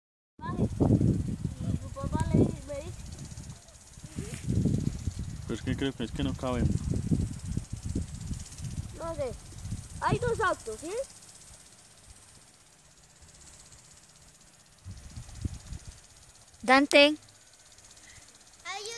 scottish terrier
disfrutando en la laguna de tota boyaca